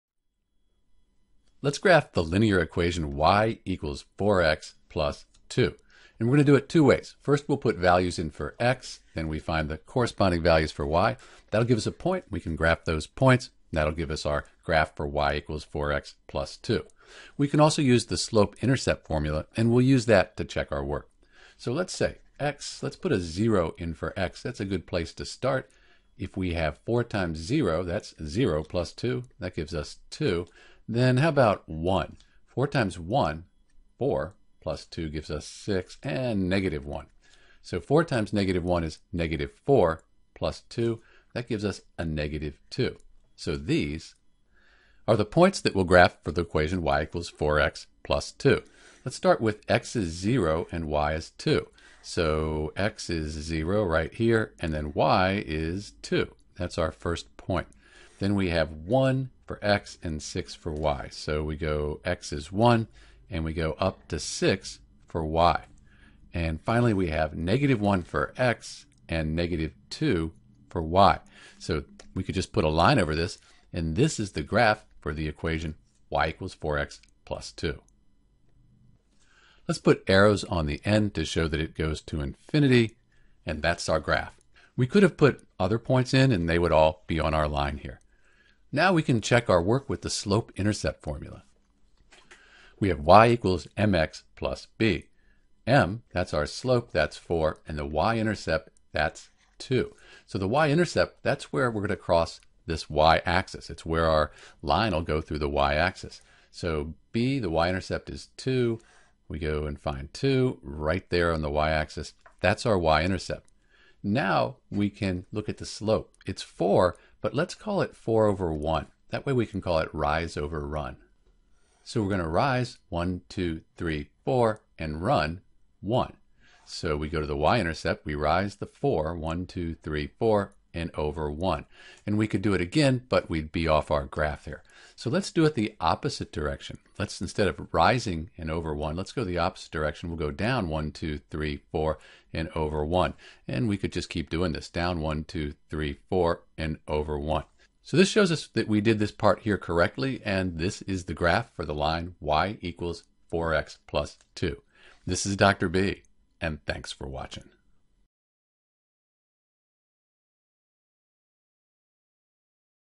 How to Graph y = 4x + 2
First, we will use a table of values to plot points on the graph. Once we have two or three points, we can draw our line for the linear equation.  You can choose any values of x for your table, however, 0, 1, and -1 are often best since they result in point that will fit on your graph paper.

m is the slope.  We can use the slope to find the rise over run.  For whole numbers, like 4, we consider that to be 4/1.  So we go up 4 on the y axis and over 1 on the x axis.  We can do that as many times as we need to in order to draw an accurate line.

I prefer to use the slope intercept formula since it is much quicker with some practice.  Then I’ll plug some values into y = 4x + 2 to make sure they fall on the line.  If they don’t there is likely a problem.

Note that this equation can also be written as y - 2  = 4x  or as 4x = y -2.  These are the same as y = 4x + 2.  They are just arranged differently.  When graphing equations it is helpful to have them in slope intercept form (y = mx + b),  just like we have in this video.